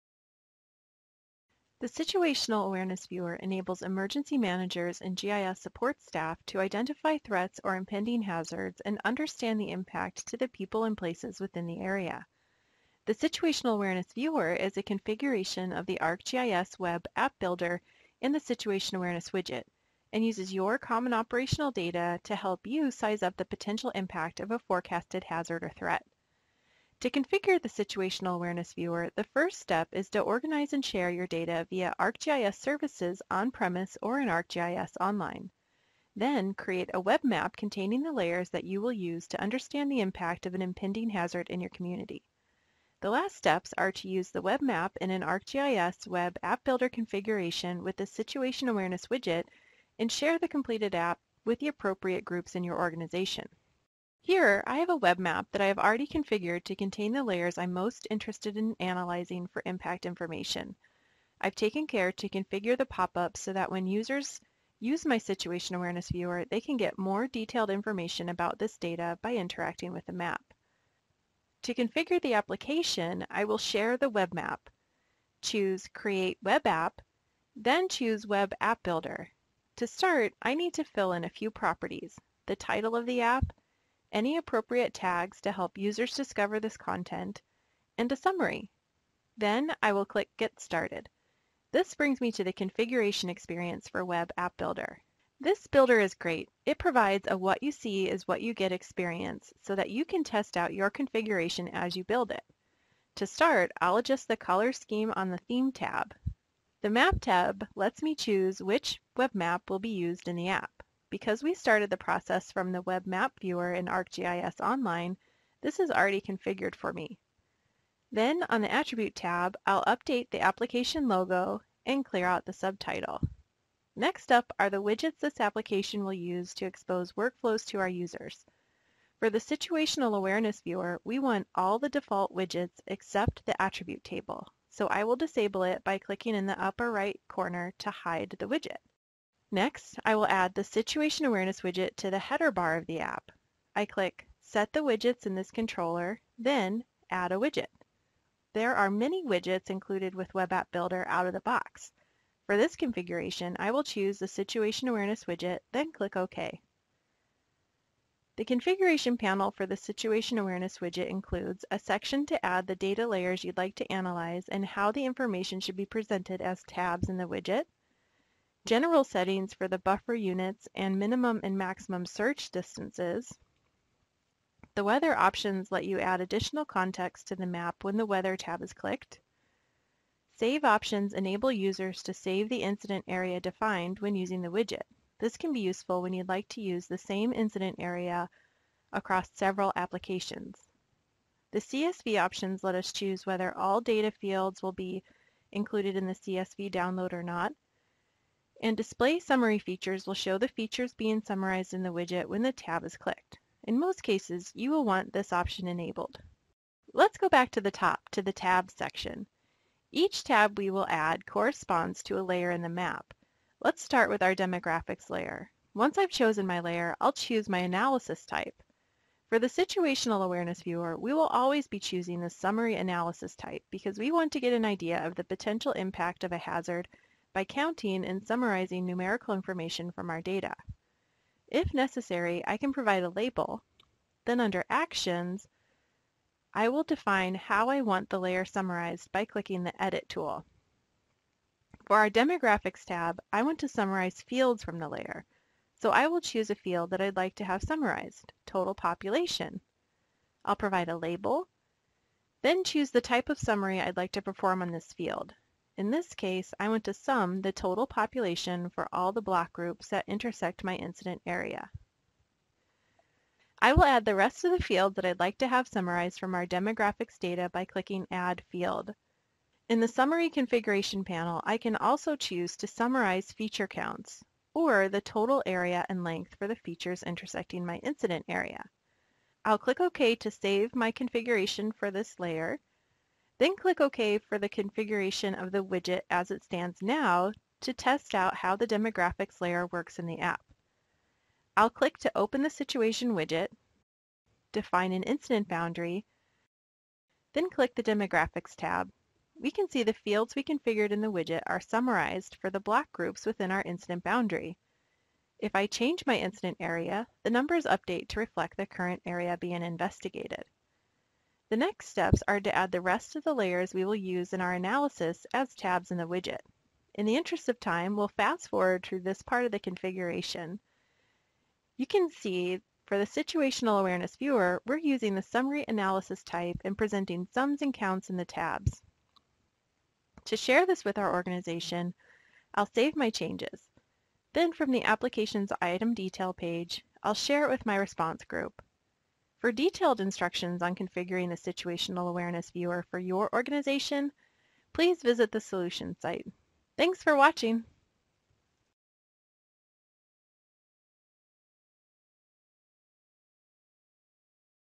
Configuring the Situational Awareness Viewer
The Situational Awareness Viewer is a configuration of Web AppBuilder that can be used to analyze the impact to people and places within an incident area. The application can be configured within ArcGIS Online or deployed on-premises.

This video provides an overview on how to configure the Situational Awareness Viewer for your organization. For more information and step-by-step instructions, please